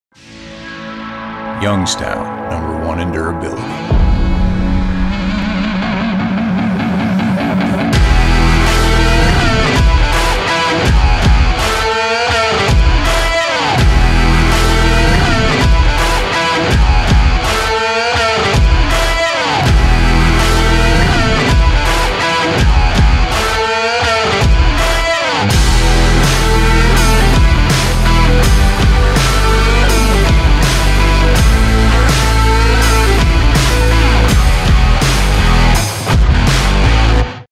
Ground Glove
Youngstown's Ground Glove is made of high quality goatskin leather and sewn into an ergonomic, 3D form fit pattern. A second layer of goatskin is sewn onto the fingertips, palm, thumb and knuckle to further enhance the durability and protection while maintaining high dexterity.

Wear Trials have shown the glove to last a Utility Field worker and average of 3 months, roughly 5x longer than traditional work gloves commonly used. Consequently, wastage, inventory management and supply chain demand is greatly reduced. Annual costs savings on work glove expenditure can be reduced by up to 30%.

The result is increased efficiency, productivity and safety for workers, while in compliance with the NFPA 70e-2015 standard and OSHA 29 CFR Part 1926.

A portion of the sale of this glove goes directly to the Fallen Linemen Organization. The FLO’s mission is to memorialize fallen electrical line workers, and care for the families who have lost or are impacted by a severe injury of a loved one in the line of duty.